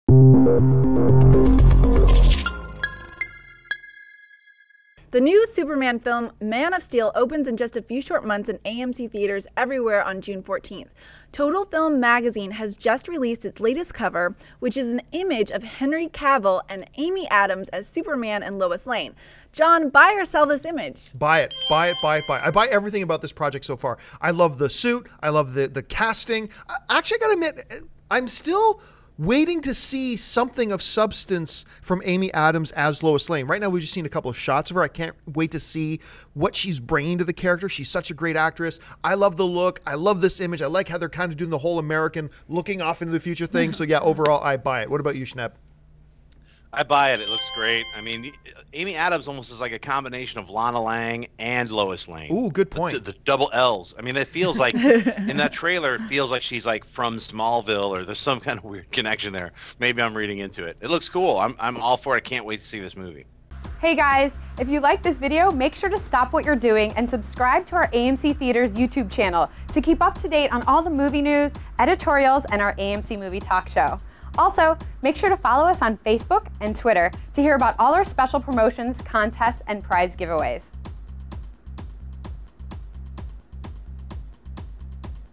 New MAN OF STEEL Photo with Lois Lane - AMC Movie News
A new magazine cover has been revealed showing a picture of Henry Cavill as Superman and Amy Adams as Lois Lane